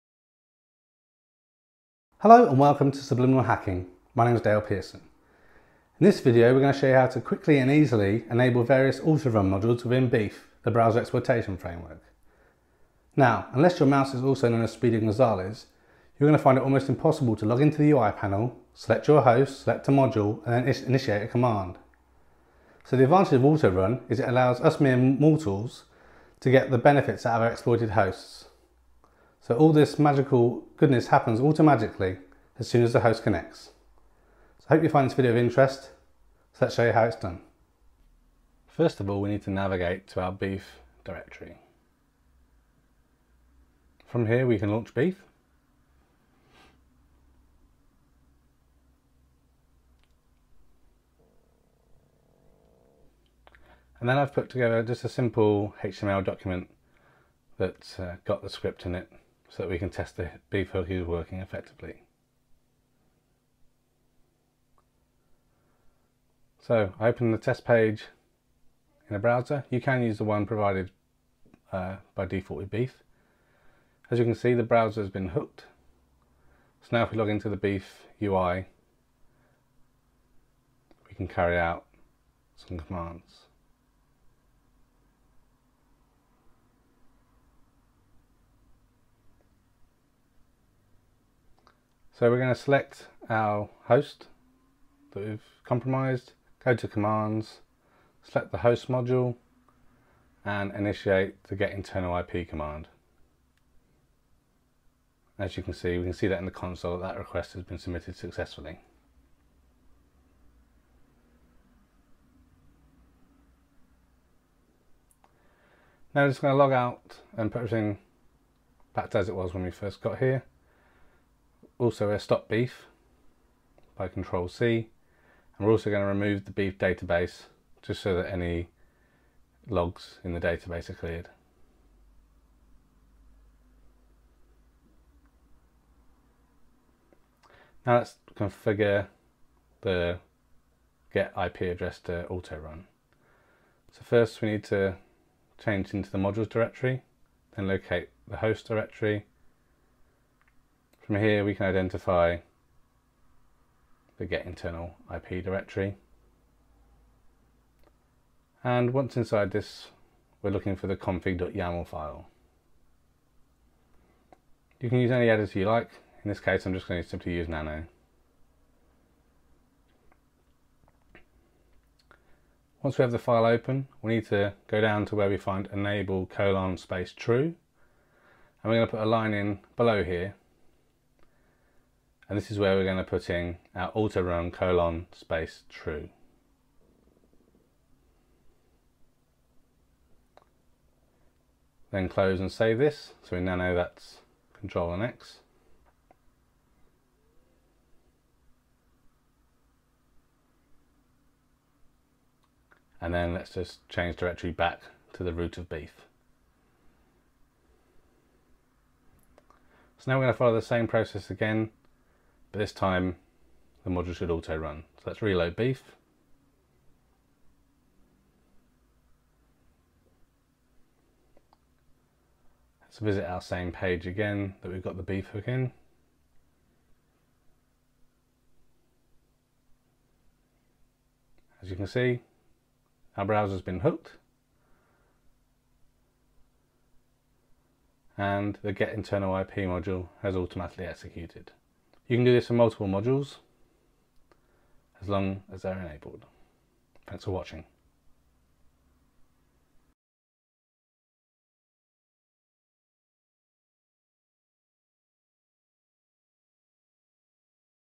How To Autorun Modules in BeEF by Subliminal Hacking
This video provides a quick How To on enable autorun on modules in  BeEF (Browser Exploitation Framework in Ubuntu.

Find out more about the Subliminal Hacking Art and Science of Social Engineering